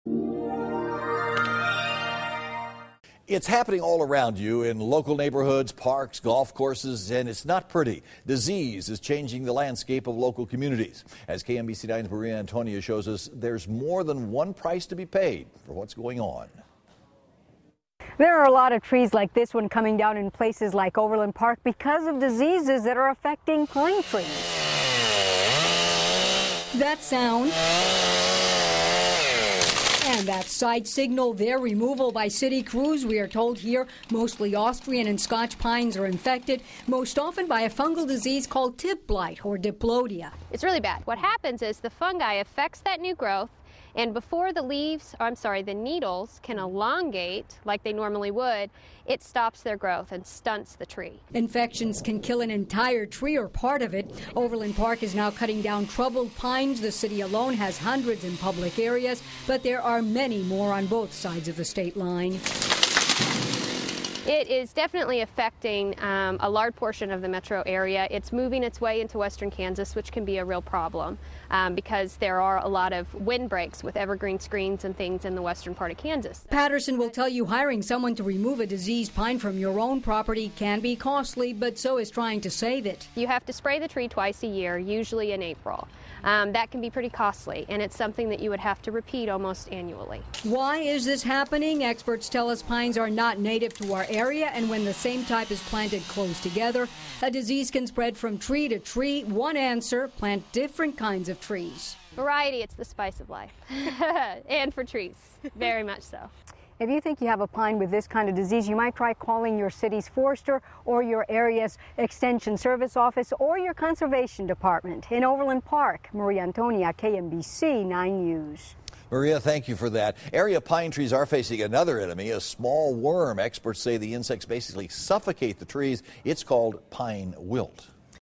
Fungus Killing Area Pine Trees
Diseases are affecting hundred of trees just in Overland Park, and many more in communities both in Kansas and Missouri. KMBC's Maria Antonia has the story.